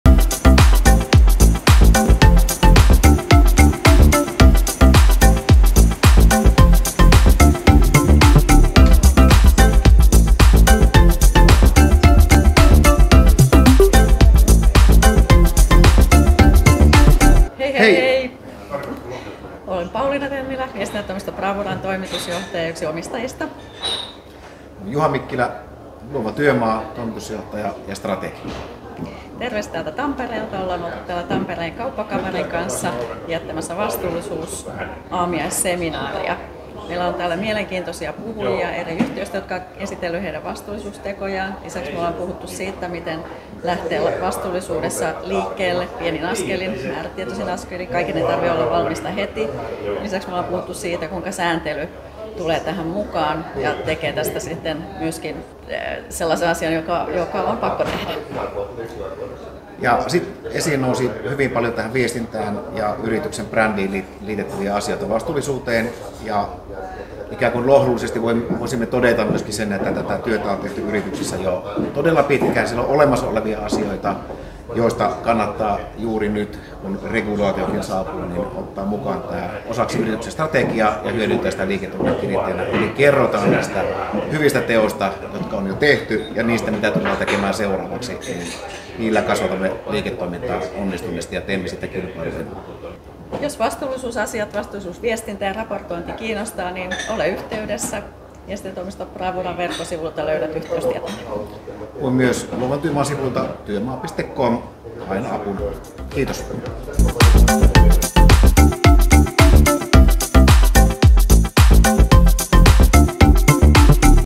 Kauppakamariaamiainen 3.2. Bravura & Luova Työmaa kooste
3.2. Kauppakamariaamiaisella Viestintätoimisto Bravura ja Luova Työmaa sekä joukko yrityksiä kertoivat, kuinka saada yritykselle parhaat tehot irti vastuullisuudesta ja sen viestinnästä.

Kiitos osallistuneille! Katso tulevat aamiaiset ja ilmoittaudu mukaan täällä